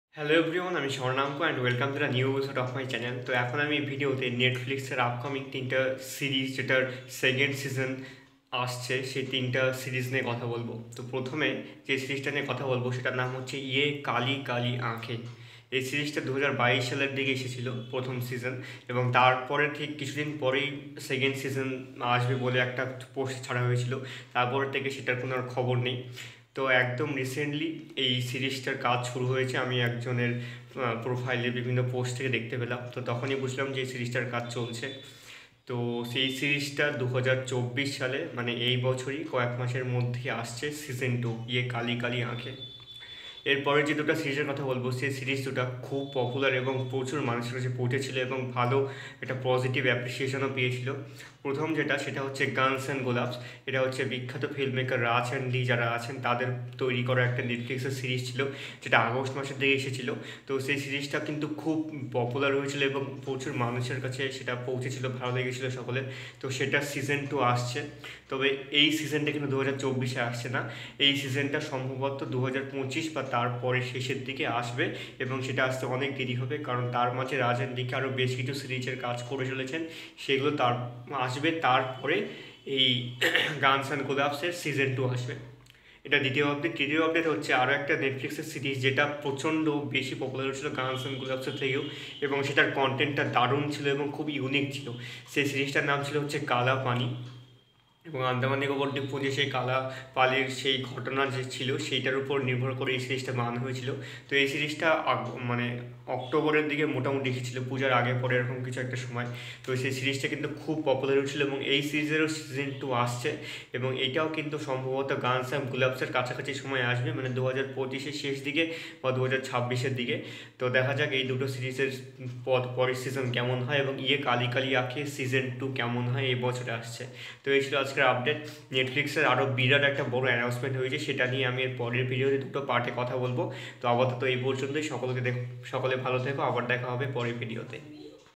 Season 2 Coming Soon ✌️🔥| Yeh Kaali Kaali Ankhein | Kaala Paani | Guns & Gulaabs | Netflix ❤️
Here I Am Presenting You News About Upcoming New Season Of 3 Netflix Series.